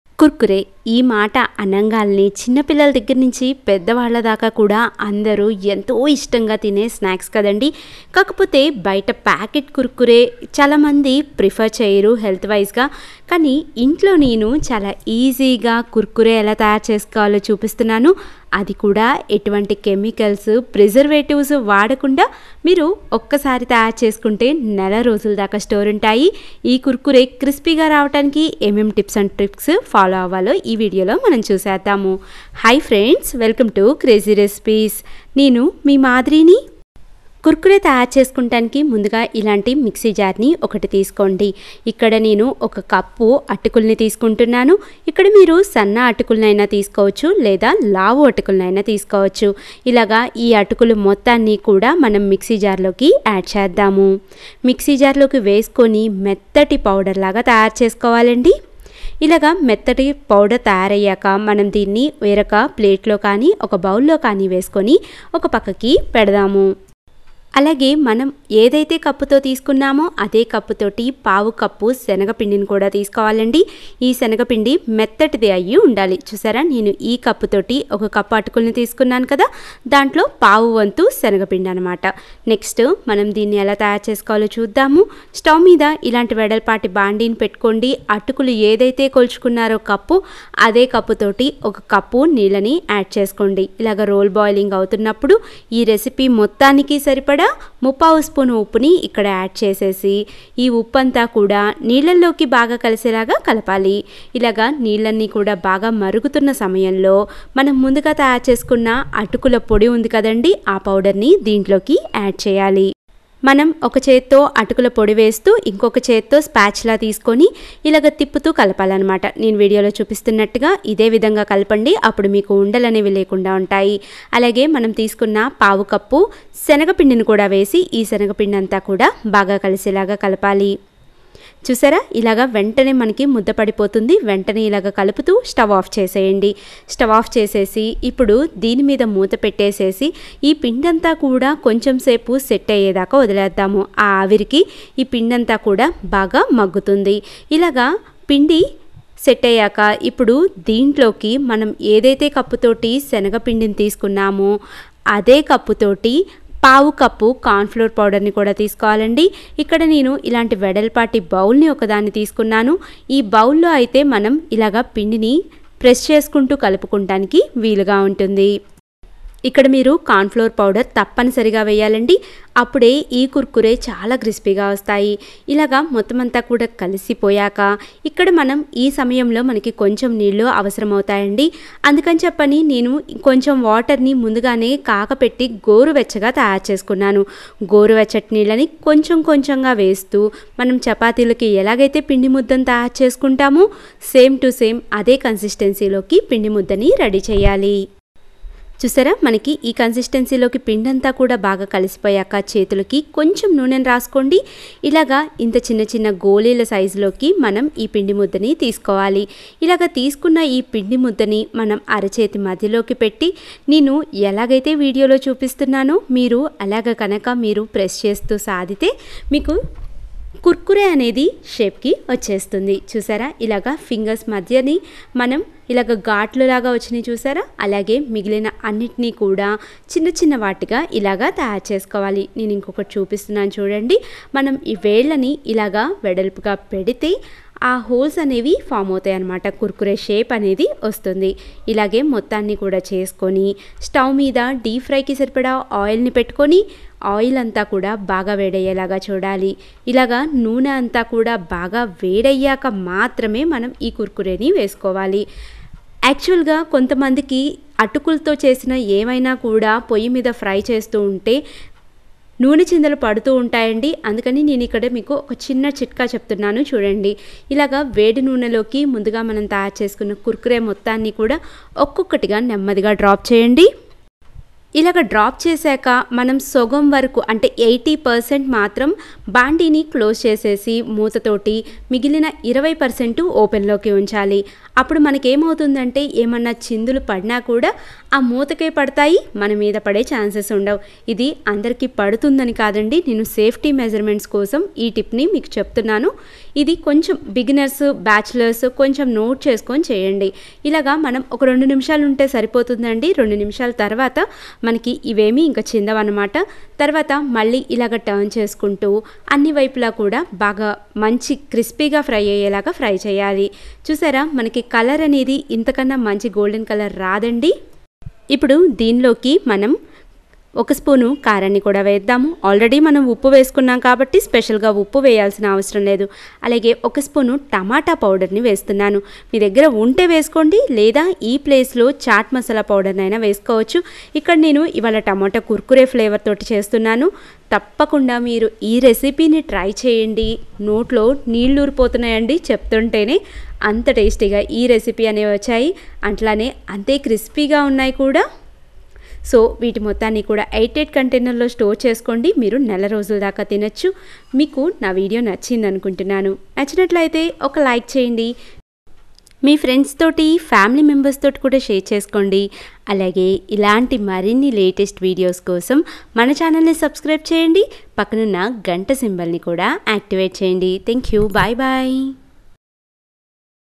కుర్కురే కేవలం 10 నిమిషాల్లో ఇంట్లోనే చెయ్యచ్చు | How to make Kurkure at home | Spicy Kurkure Recipe
కుర్కురే కేవలం 10 నిమిషాల్లో ఇంట్లోనే చెయ్యచ్చు | Kurkure Within 10min || How to Make Kurkure

Kurkure Kurkure recipe
kurkure banane ka tarika
kurkure kaise banaye
kurkure banane ki vidhi
kurkure kurkure